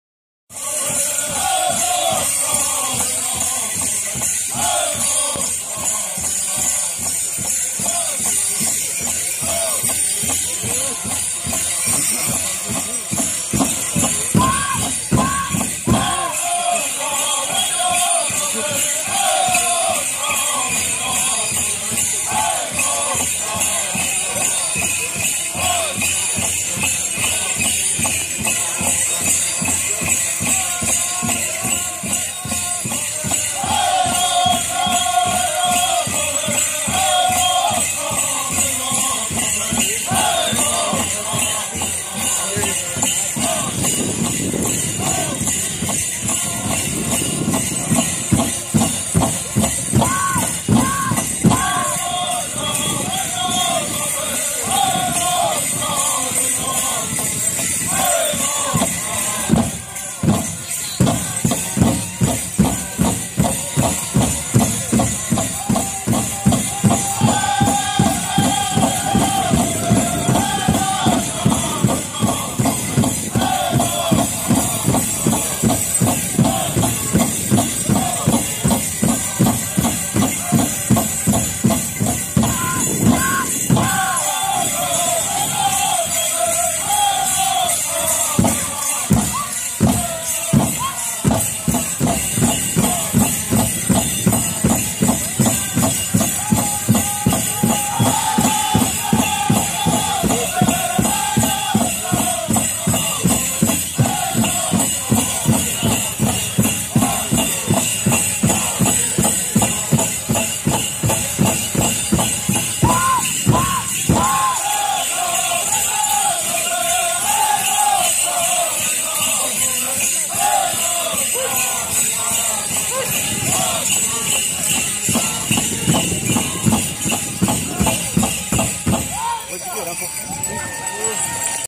Plumley Straight special- 1st song-at Stroud 2015
This is the first song for the first group of the Earl Plumley Straight dance contest at Sac & Fox powwow in Stroud, Oklahoma on July 10, 2015.